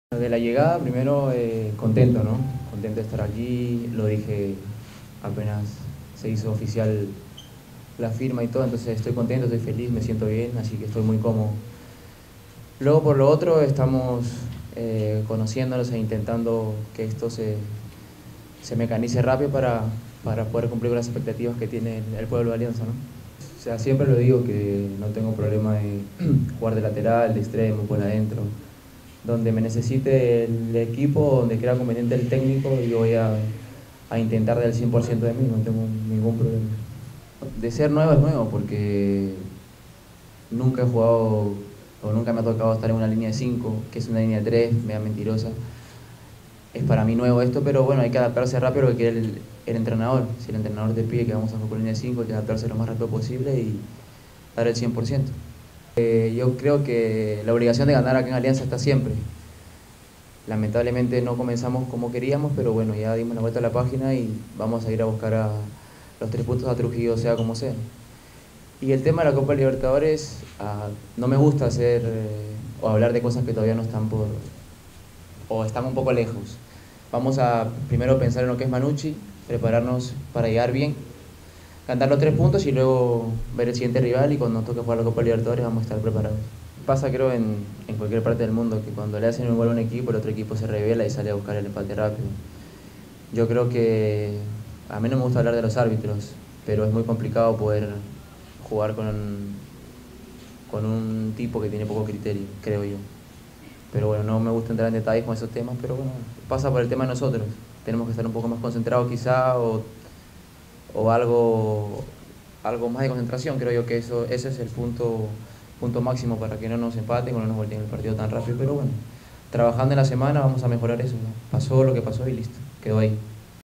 Alexi Gómez "Estamos corrigiendo nuestros errores"
Jugador Alianza LIma